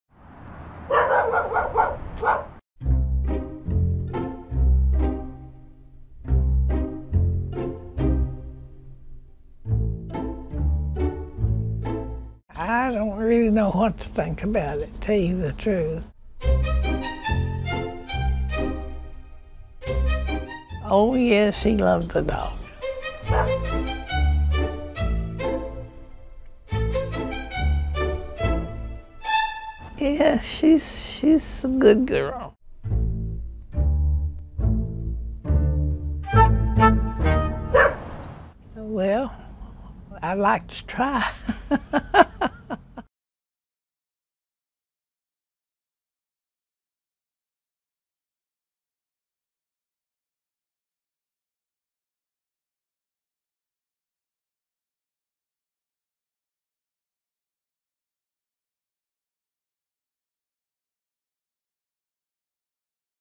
Dog inherits $5 million from deceased owner - BBC News
Lulu the border collie was left $5 million (£3.6 million) after her owner died last year. Bill Dorris left the dog in the care of his friend, Martha Burton. The will states that Burton is to be reimbursed for Lulu's reasonable monthly expenses.